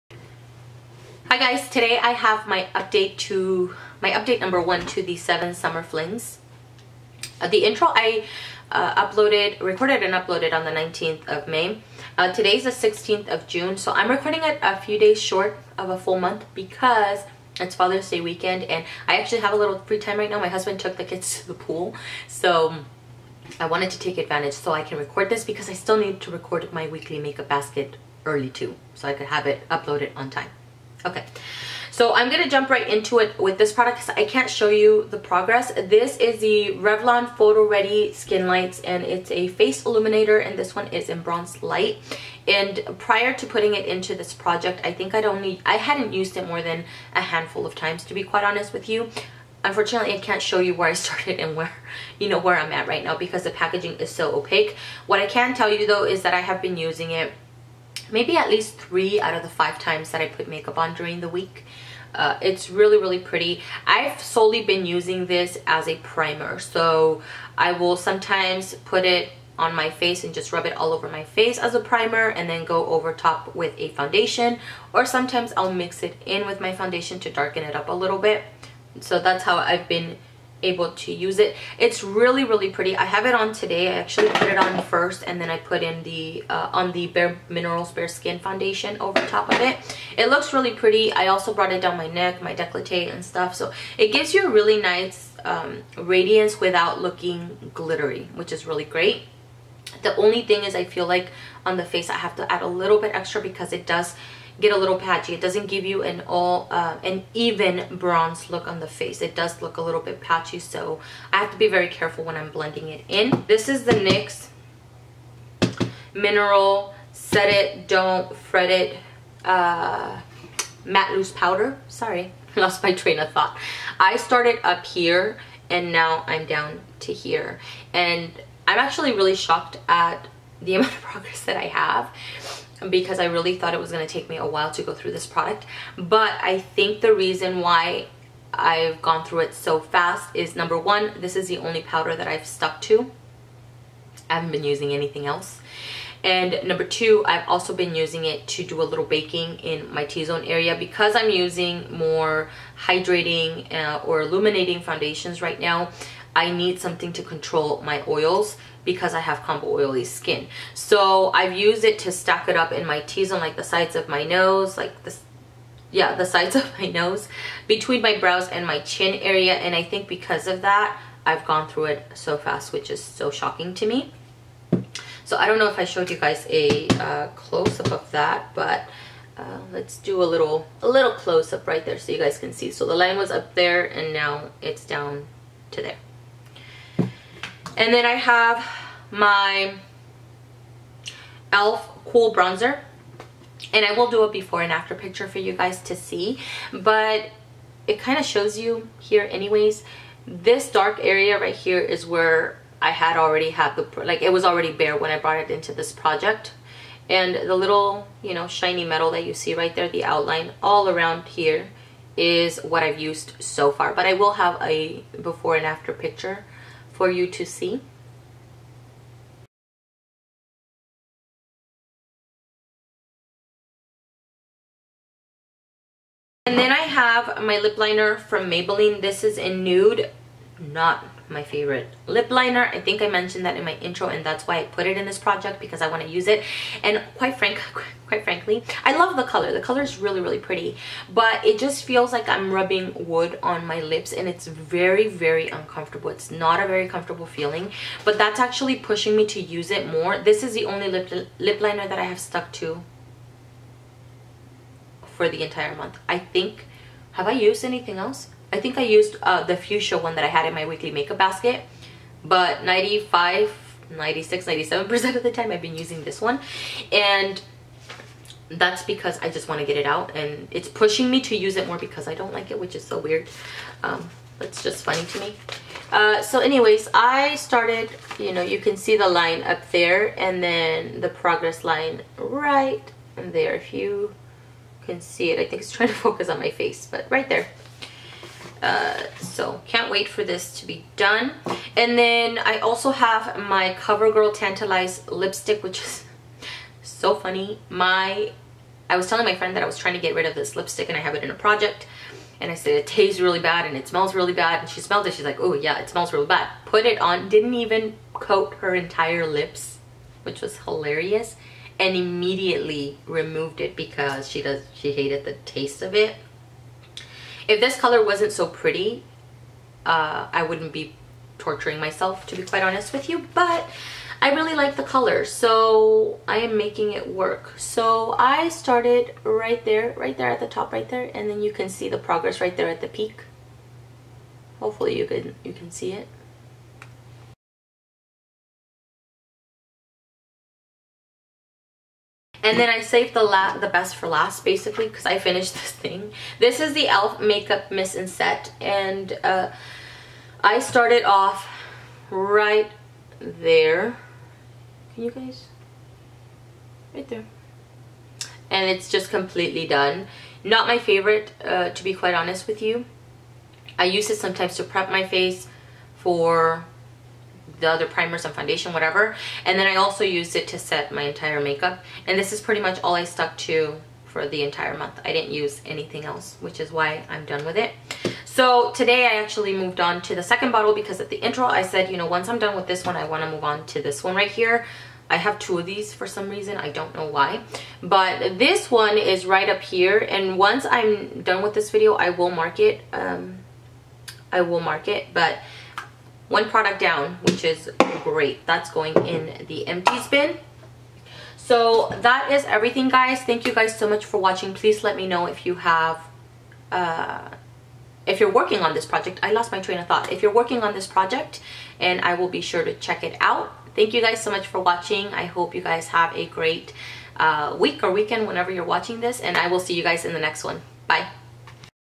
7 Summer Flings Update 1
Hello Hello,
hope you are all having a blessed Friday.  Its update time for the items I am trying to finish throughout summer.  Please let me know if you are doing thing project, I would love to follow along.

I forgot to talk about the NYX pore filler, but did include a picture of it.  I have been applying it on a regular basis but don't need much of it.  I find when I over apply my makeup moves around.  For the most part I only apply it where I have larger pores which is in my T-zone area.

Diana